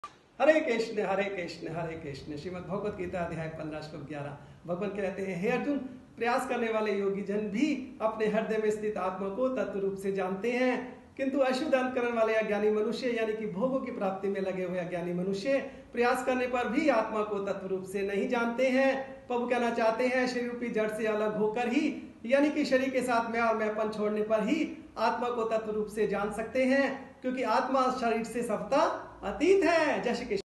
🙏Gita 11/15🙏Those engaged in getting pleasures don’t know soul in its true form even after tryiing🙏
🙏Srimad Bhagavad Gita Chapter 15 Verse 11🙏
People with impure hearts (engaged in the pursuit of pleasures) do not know the soul in its true form even after trying.

🙏Hare Krishna🙏